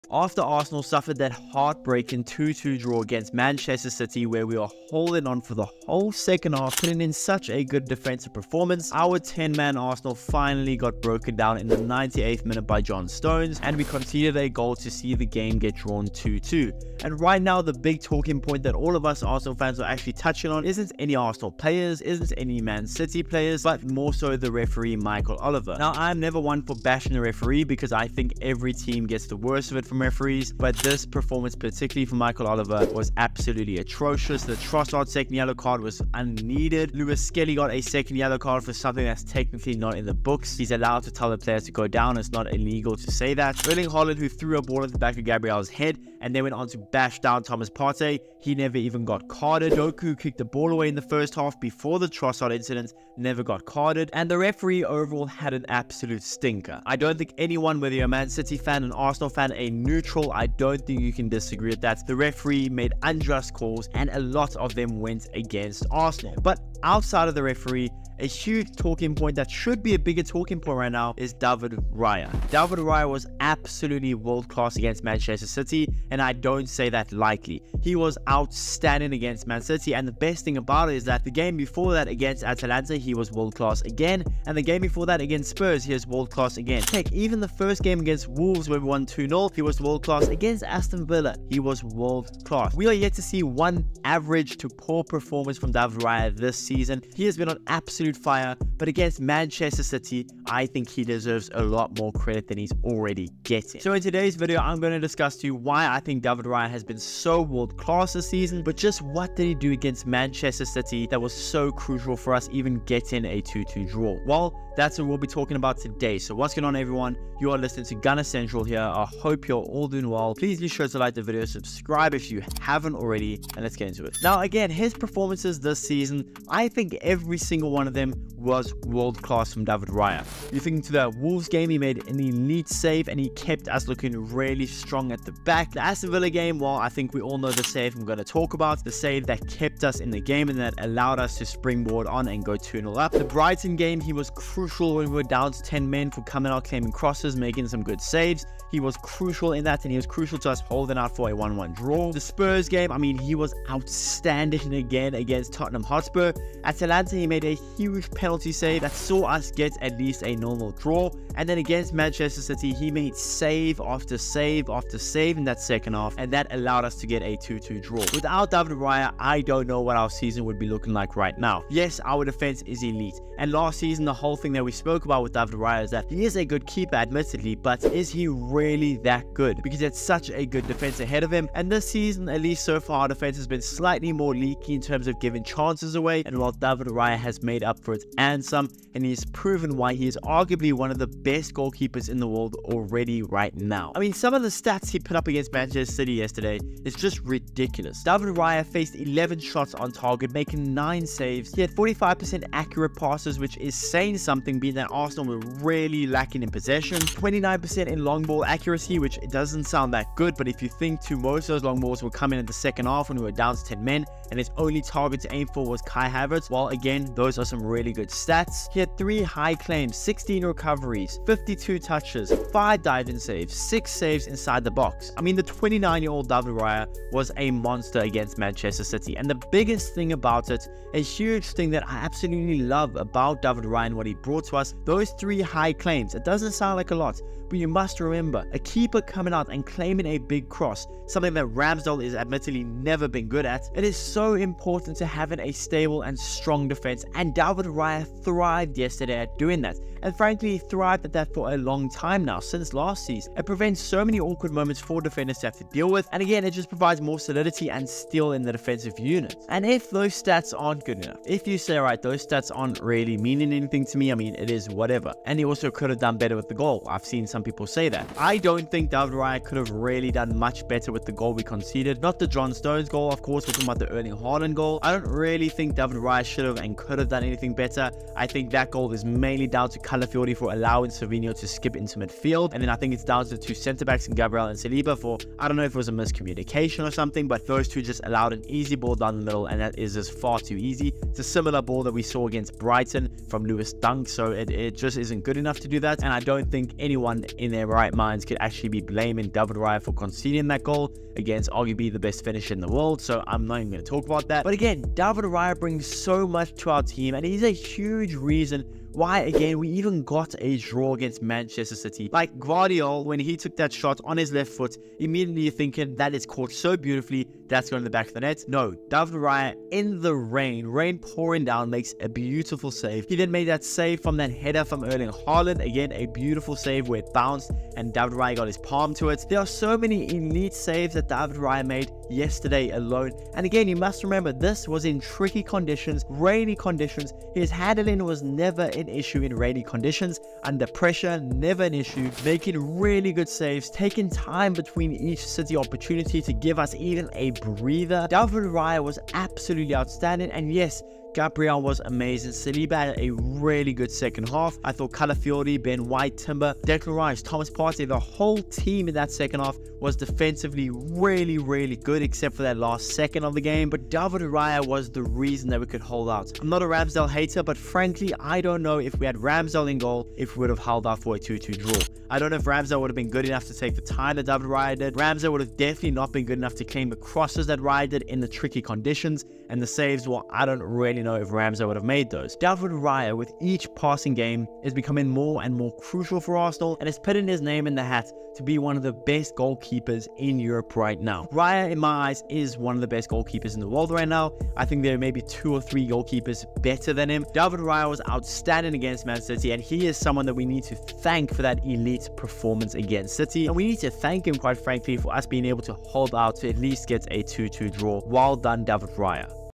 David Raya Was WORLD CLASS Against Man City
arsenal mancity raya Discussing David Raya's performance and amazing saves in Arsenal's 2-2 draw against Manchester City in the Etihad Stadium.